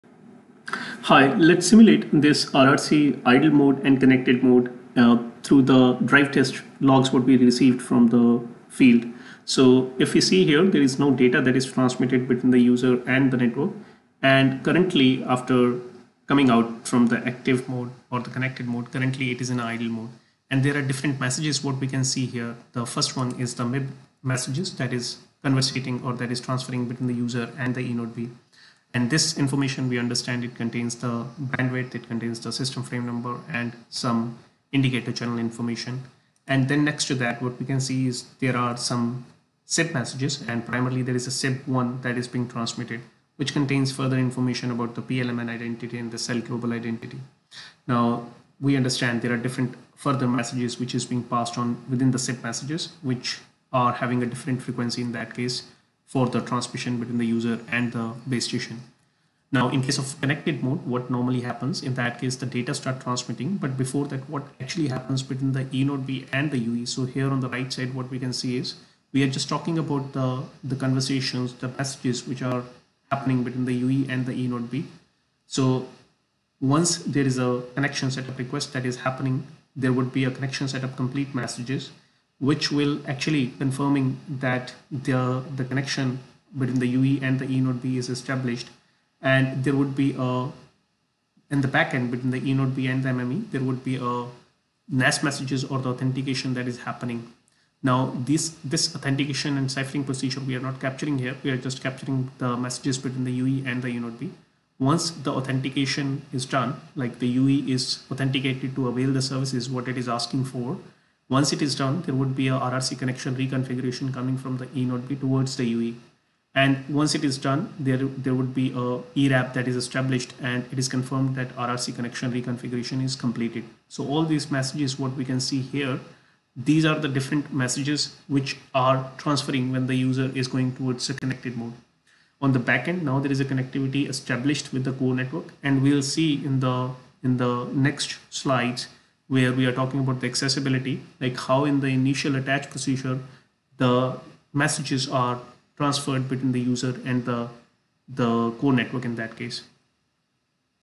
Layer 3 - RRC Idle & connected (Simulation using drive test)
RRC – Radio resource control protocol exists at IP level (layer 3) between UE and eNB.

The major functions of the RRC protocol include connection establishment and release functions, broadcast of system information, radio bearer establishment, reconfiguration and release, RRC connection mobility procedures, paging notification and release and outer loop power control.

The operation of the RRC is guided by a state machine which defines certain specific states that a UE may be present in. The different states in this state machine have different amounts of radio resources associated with them and these are the resources that the UE may use when it is present in each specific state.

1.RRC_IDLE:

•PLMN selection

•Broadcast of system information

•Cell re-selection mobility

•Paging for mobile terminated data is initiated by EPC

•DRX for CN paging configured by NAS

2. RRC_CONNECTED:

•EPC – RAN connection (both C/U-planes) is established for UE

•The UE AS context is stored in RAN and the UE

•RAN knows the cell which the UE belongs to

•Transfer of unicast data to/from the UE

•Network controlled mobility including measurements